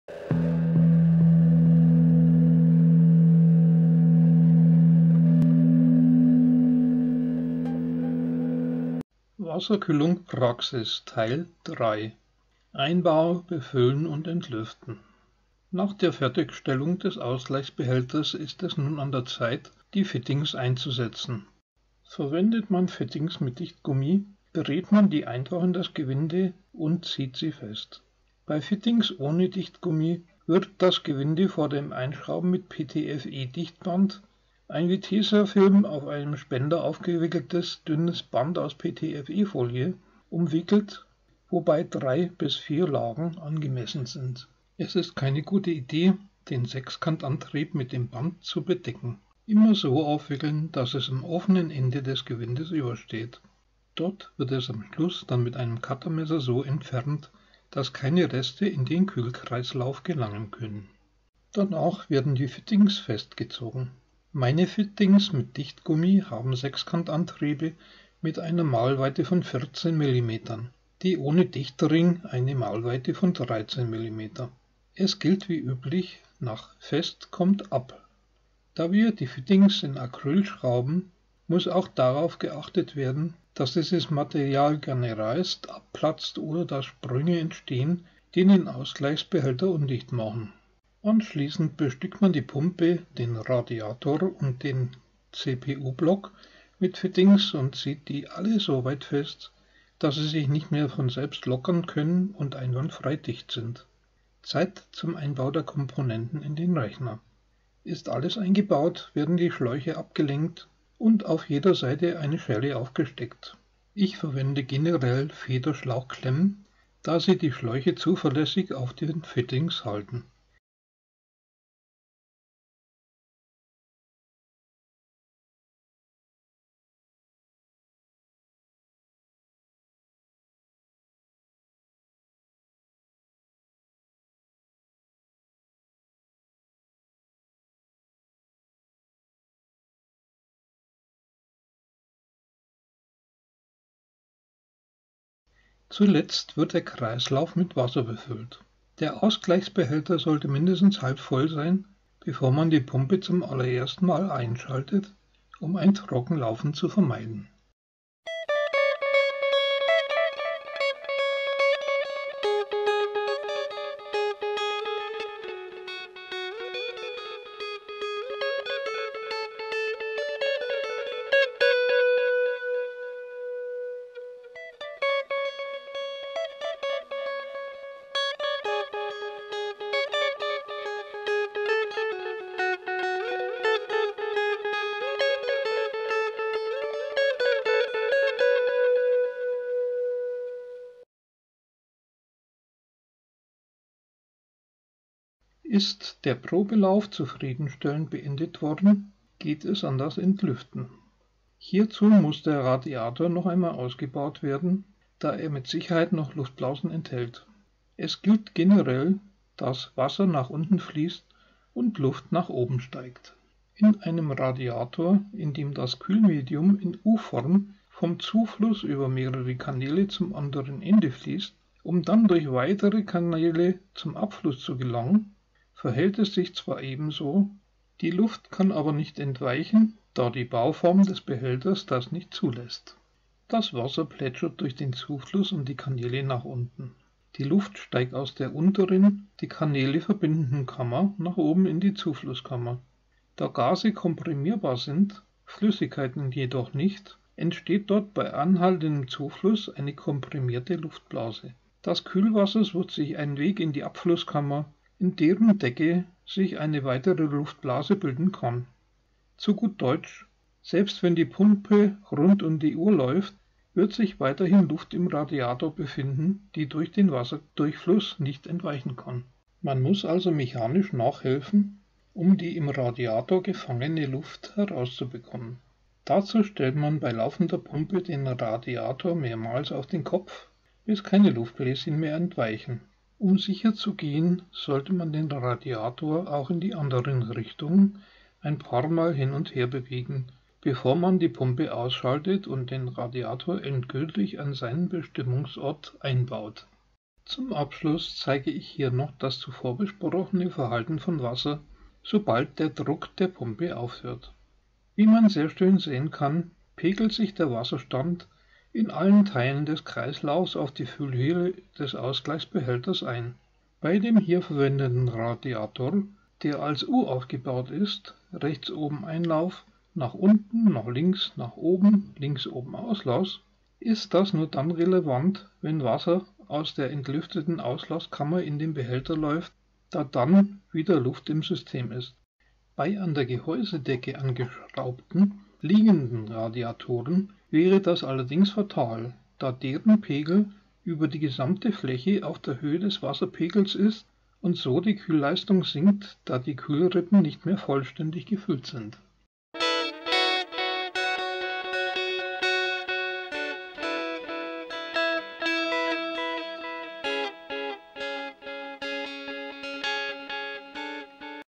Wasserkühlung in der Praxis - Teil 3: Einbau, Befüllen und Entlüften
Nach der Fertigstellung des Ausgleichsbehälters ist es nun an der Zeit, die Fittings einzusetzen. Verwendet man Fittings mit Dichtgummi, dreht man die einfach in das Gewinde und zieht sie fest. Bei Fittings ohne Dichtgummi wird das Gewinde vor dem Einschrauben mit PTFE-Dichtband - ein wie Tesafilm auf einem Spender aufgewickeltes, dünnes Band aus PTFE-Folie - umwickelt, wobei drei bis vier Lagen angemessen sind. Es ist keine gute Idee, den Sechskantantrieb mit dem Band zu bedecken - immer so aufwickeln, dass es am offenen Ende des Gewindes übersteht. Dort wird es am Schluss dann mit einem Cuttermesser so entfernt, dass keine Reste in den Kühlkreislauf gelangen können.

Danach werden die Fittings festgezogen. Meine Fittings mit Dichtgummi haben Sechskantantriebe mit einer Maulweite von 14 mm, die ohne Dichtring eine Maulweite von 13 mm. Es gilt wie üblich: „Nach fest kommt ab!“ Da wir die Fittings in Acryl schrauben, muss auch darauf geachtet werden, dass dieses Material gern reißt, abplatzt oder dass Sprünge entstehen, die den Ausgleichsbehälter undicht machen.

Anschließend bestückt man die Pumpe, den Radiator und den CPU-Block mit Fittings, und zieht die alle so weit fest, dass sie sich nicht mehr von selbst lockern können und einwandfrei dicht sind. Zeit zum Einbau der Komponenten in den Rechner. Ist alles eingebaut, werden die Schläuche abgelängt, und auf jeder Seite eine Schelle aufgesteckt. Ich verwende generell Federschlauchklemmen, da sie die Schläuche zuverlässig auf den Fittings halten. Zuletzt wird der Kreislauf mit Wasser befüllt. Der Ausgleichsbehälter sollte mindestens halb voll sein, bevor man die Pumpe zum allerersten Mal einschaltet, um ein Trockenlaufen zu vermeiden.

Ist der Probelauf zufriedenstellend beendet worden, geht es an das Entlüften. Hierzu muss der Radiator noch einmal ausgebaut werden, da er mit Sicherheit noch Luftblasen enthält. Es gilt generell, dass Wasser nach unten fließt und Luft nach oben steigt. In einem Radiator, in dem das Kühlmedium in U-Form vom Zufluss über mehrere Kanäle zum anderen Ende fließt, um dann durch weitere Kanäle zum Abfluss zu gelangen, verhält es sich zwar ebenso, die Luft kann aber
nicht entweichen, da Bauform des Behälters das nicht zulässt.

Das Wasser plätschert durch den Zufluss und die Kanäle nach unten. Die Luft steigt aus der unteren, die Kanäle verbindenden Kammer nach oben in die Zuflusskammer. Da Gase komprimierbar sind, Flüssigkeiten jedoch nicht, entsteht dort bei anhaltendem Zufluss eine komprimierte Luftblase. Das Kühlwasser sucht sich einen Weg in die Abflusskammer, in deren Decke sich eine weitere Luftblase bilden kann. Zu gut Deutsch - selbst wenn die Pumpe rund um die Uhr läuft, wird sich weiterhin Luft im Radiator befinden, die durch den Wasserdurchfluss nicht entweichen kann.

Man muss also mechanisch nachhelfen, um die im Radiator „gefangene“ Luft herauszubekommen. Dazu stellt man bei laufender Pumpe den Radiator mehrmals auf den Kopf, bis keine Luftbläschen mehr entweichen. Um sicherzugehen, sollte man den Radiator auch in die anderen Richtungen ein paar Mal hin und her bewegen, bevor man die Pumpe ausschaltet und den Radiator endgültig an seinem Bestimmungsort einbaut.

Zum Abschluss zeige ich hier noch das zuvor besprochene Verhalten von Wasser, sobald der Druck der Pumpe aufhört. Wie man sehr schön sehen kann, pegelt sich der Wasserstand in allen Teilen des Kreislaufs auf die Füllhöhe des Ausgleichsbehälters ein. Bei dem hier verwendeten Radiator, der als U aufgebaut ist - rechts oben Einlauf, nach unten, nach links, nach oben, links oben Auslass - ist das nur dann relevant, wenn Wasser aus der entlüfteten Auslasskammer in den Behälter läuft, da dann wieder Luft im System ist. Bei an der Gehäusedecke angeschraubten liegenden Radiatoren wäre das allerdings fatal, da deren Pegel über die gesamte Fläche auf der Höhe des Wasserspiegels ist, und so die Kühlleistung sinkt, da die Kühlrippen nicht vollständig gefüllt sind.